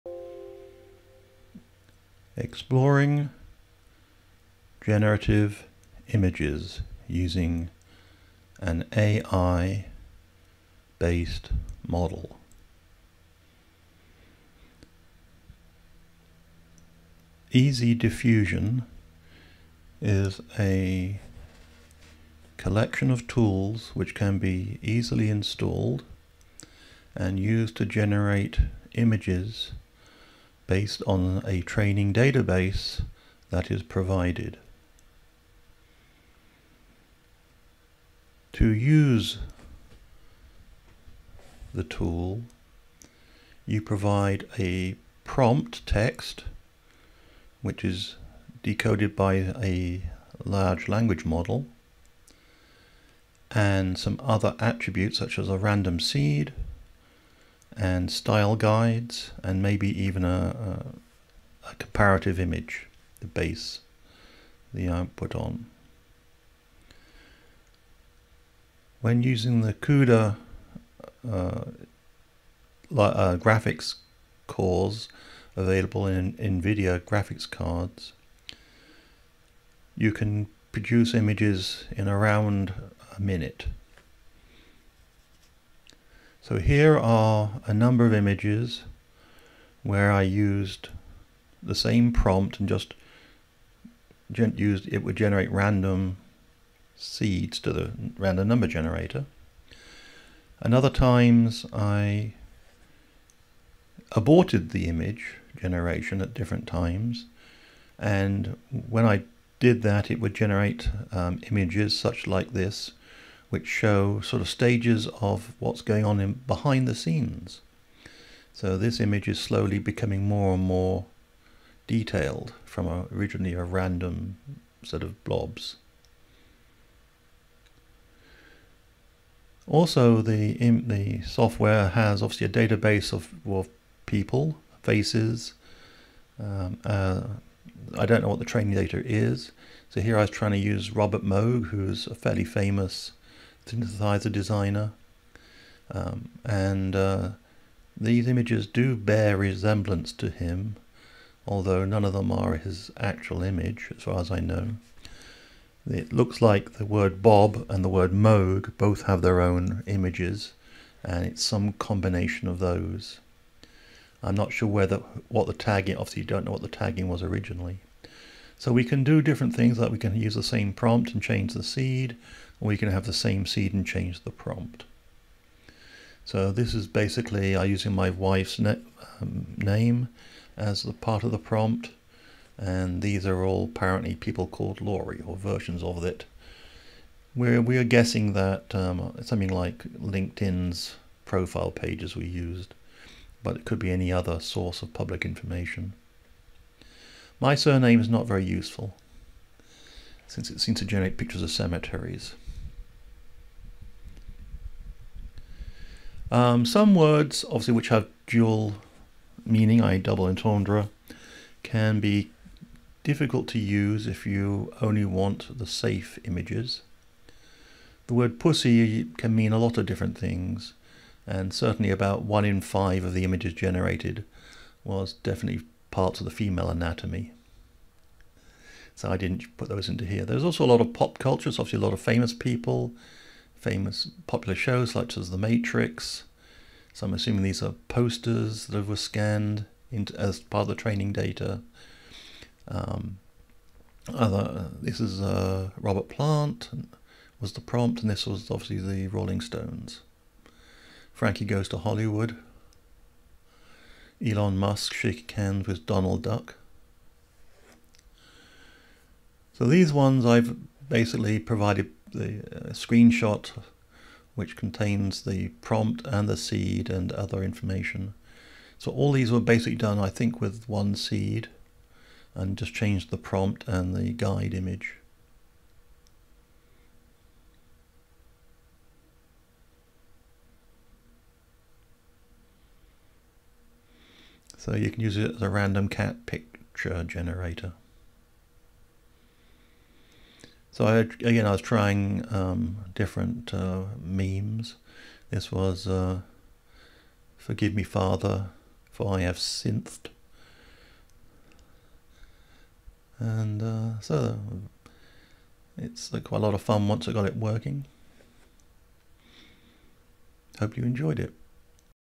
AI generative images on your PC
The images in this video were generated on my PC with an Nvidia GTX 1660 Super graphics card. Each image took around a minute to generate. Without the CUDA cores using only the CPU it took about 10 minutes per image.
I explore how changing only the prompt text or only the random seed makes the image different. By aborting the process at various times you can also get an insight into how the process works.